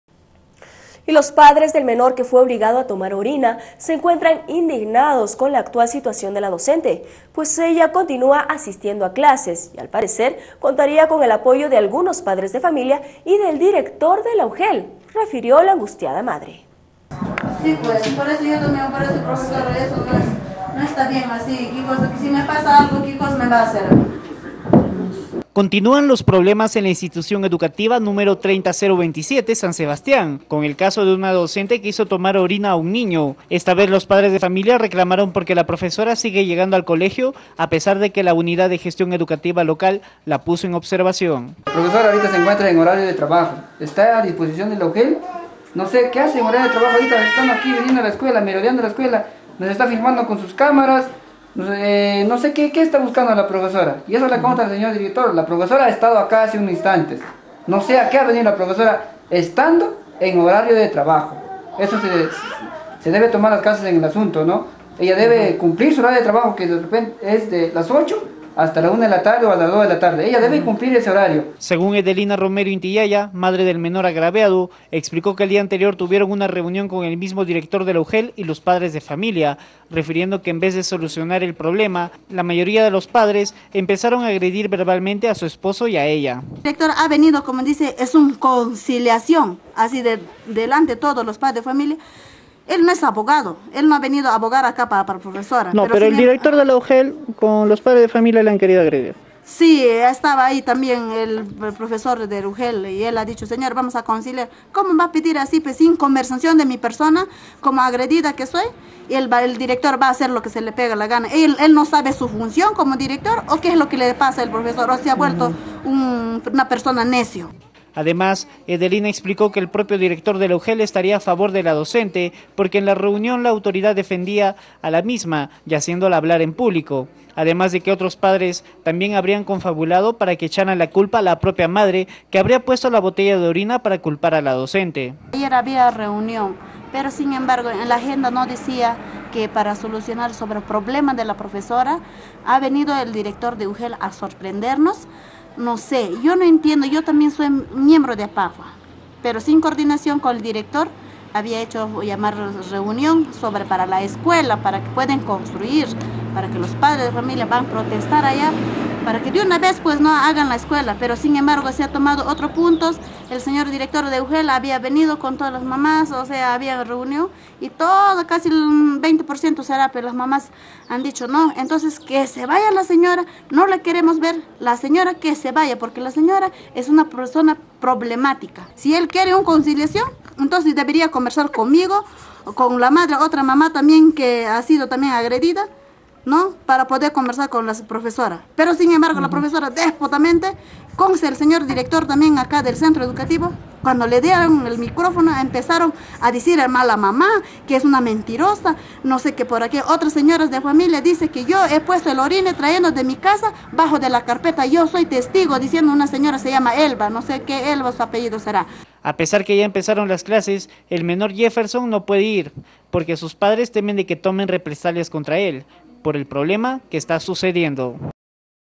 ESTARÍAN DISCRIMINANDO A DENUNCIANTE.
Ugel, refirió la angustiada madre.
Los padres del menor que fue obligado a tomar orina, se encuentran indignados con la actual situación de la docente, pues ella continua asistiendo a clases y al parecer contaría con el apoyo de algunos padres de familia y del Director de la